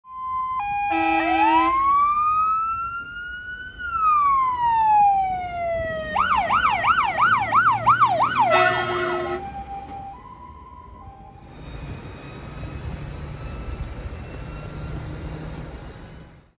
Alarmowo do pożaru samochodu: 305[P]21 GBA z JRG5 Poznań
Do pożaru doszło wczoraj na terenie parkingu należącego do galerii „Pestka” w Poznaniu.